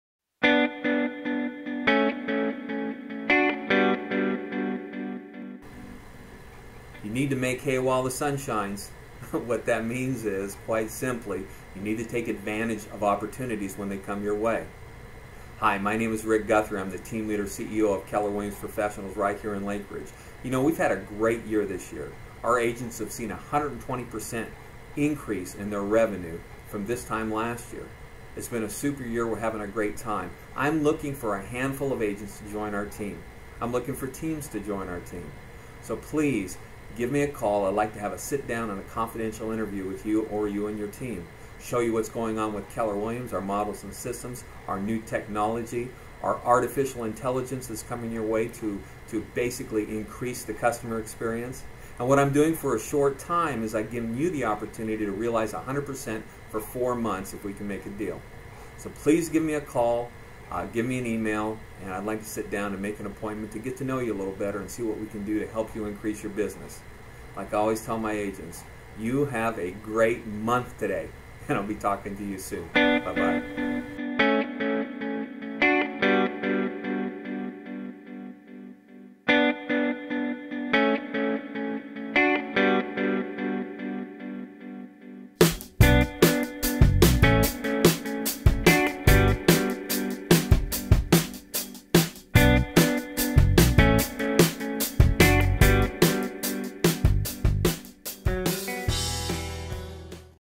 Make Hay While The Sun Shines
Rick Guthrie, Team Leader of Keller Williams Professionals Lake Ridge, is looking for talented agents and brokers to join our KW family. For a limited time he's making a great offer to agents that are a great fit for this incredible office which has experienced over a 120% increase in productivity year to date from last year using Keller Williams proven systems and Models.